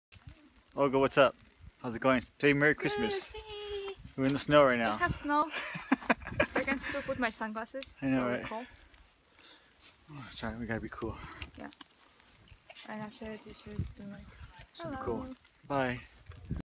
play merry xmas
We got snowed on while out bouldering in bishop.